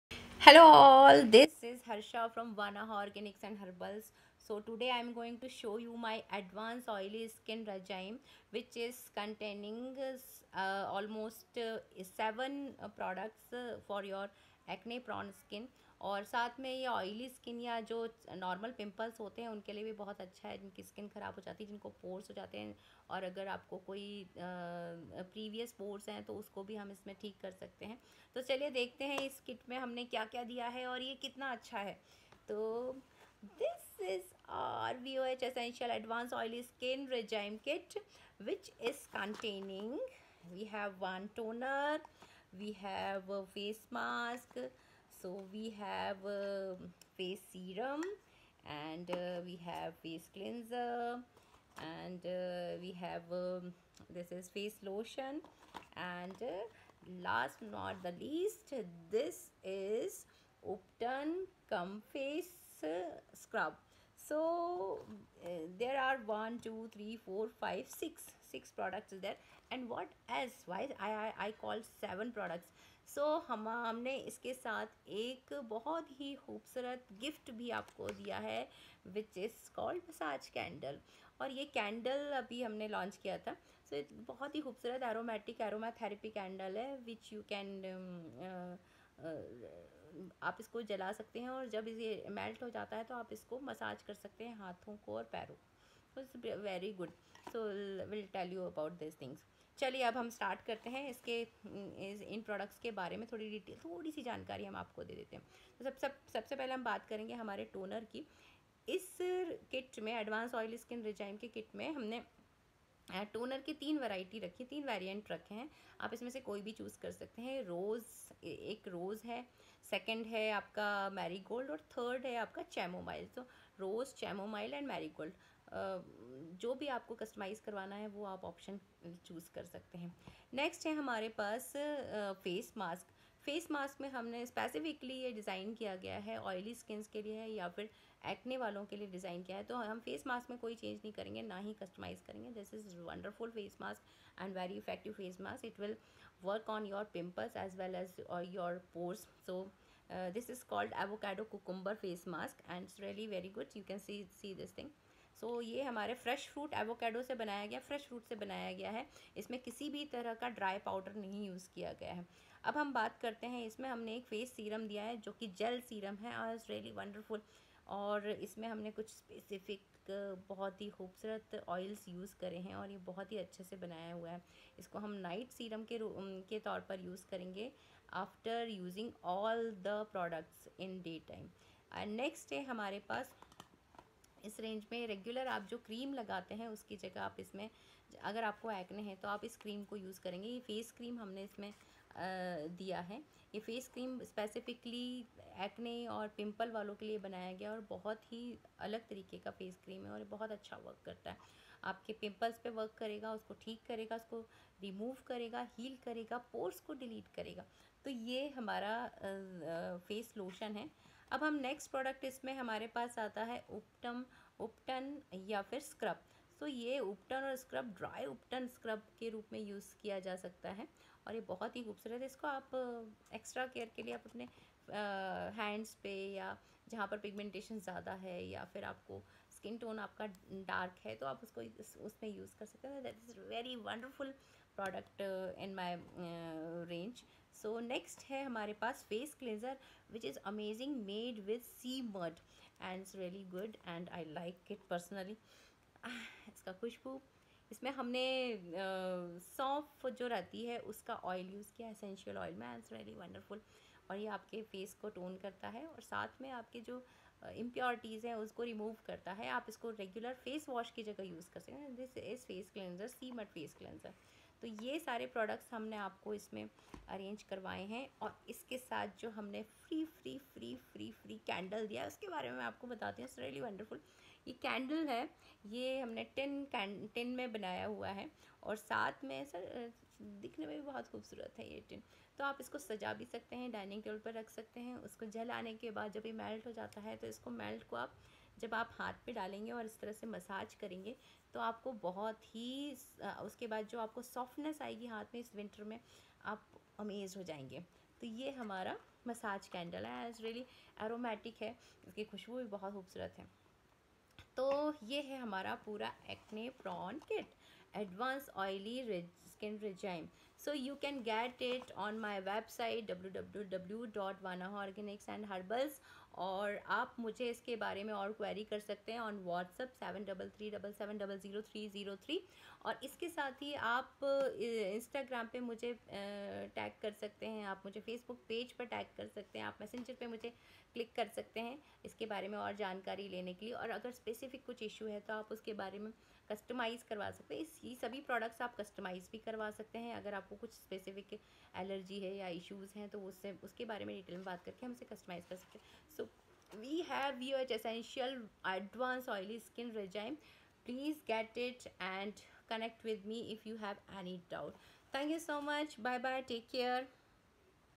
Advance Oily Skin care regimen
This kit is specially designed for a cute acne ,oily and pimples skin. This advance oily skin regimen containing 6 amazing remedies to cure acne and pores
Advance Oily Skin Regime
Face Cleanser
Face scrub
Face pack
facetoner
Serum
Face lotion
We believe in tangible results you can see and feel, and we believe in the power of nature to bring these results.Inspired by traditional Indian home remedies and Herbs for skin care